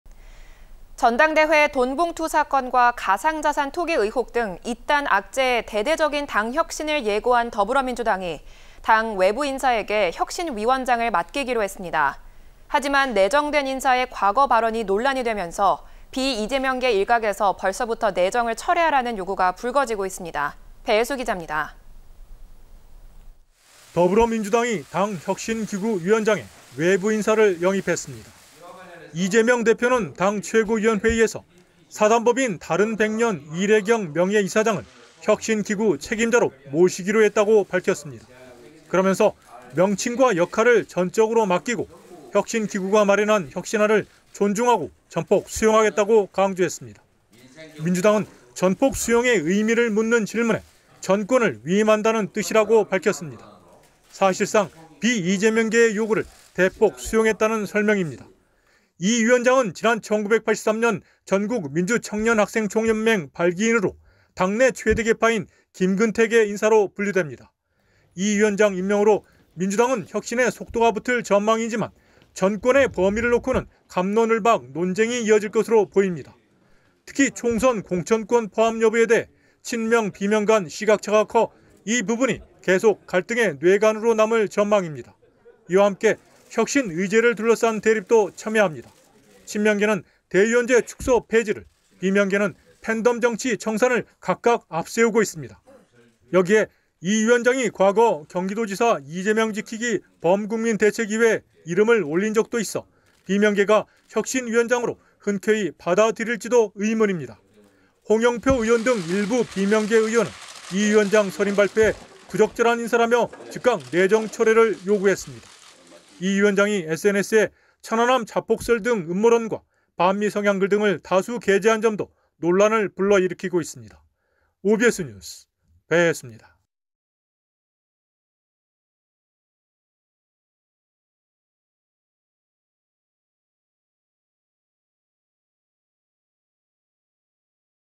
민주당 혁신기구 수장에 이래경, 과거 발언들 보니.. / OBS 경인플러스
【앵커】

전당대회 돈봉투 사건과 가상자산 투기 의혹 등 잇단 악재에 대대적인 당 혁신을 예고한 더불어민주당이 당 외부인사에게 혁신위원장을 맡기기로 했습니다.

하지만 내정된 인사의 과거 발언이 논란이 되면서 비이재명계 일각에서 벌써부터 내정을 철회하라는 요구가 불거지고 있습니다.

배해수 기자입니다.

【기자】

더불어민주당이 당 혁신기구 위원장에 외부인사를 영입했습니다.

이재명 대표는 당 최고위원회의에서 사단법인 다른백년 이래경 명예이사장을 혁신기구 책임자로 모시기로 했다고 밝혔습니다.

그러면서 "명칭과 역할을 전적으로 맡기고 혁신기구가 마련한 혁신안을 존중하고 전폭 수용하겠다"고 강조했습니다.

민주당은 '전폭 수용'의 의미를 묻는 질문에 "전권을 위임한다"는 뜻이라고 밝혔습니다.

사실상 비이재명계의 요구를 대폭 수용했다는 설명입니다.

이 위원장은 지난 1983년 전국민주청년학생총연맹(민청련) 발기인으로, 당내 최대 계파인 김근태계 인사로 분류됩니다.

이 위원장 임명으로 민주당은 혁신에 속도가 붙을 전망이지만 전권의 범위를 놓고는 갑론을박논쟁이 이어질 것으로 보입니다.

특히 총선 공천권 포함 여부에 대해 친명-비명간 시각차가 커 이 부분이 계속 갈등의 뇌관으로 남을 전망입니다.

이와함께 혁신 의제를 둘러싼 대립도 첨예합니다.

친명계는 대의원제 축소.폐지를, 비명계는 팬덤정치 청산을 각각 앞세우고 있습니다.

여기에 이 위원장이 과거 '경기도지사 이재명 지키기 범국민대책위'에 이름을 올린 적이 있어 비명계가 혁신위원장으로 흔쾌히 받아들일지도 의문입니다.

홍영표 의원 등 일부 비명계 의원은 이 위원장 선임 발표에 '부적절한 인사'라며 즉각 내정 철회를 요구했습니다.

이 위원장이 SNS에 '천안함 자폭설'등 음모론과 반미 성향 글 등을 다수 게재한 점도 논란을 불러일으키고 있습니다.

 OBS 뉴스 배해수입니다.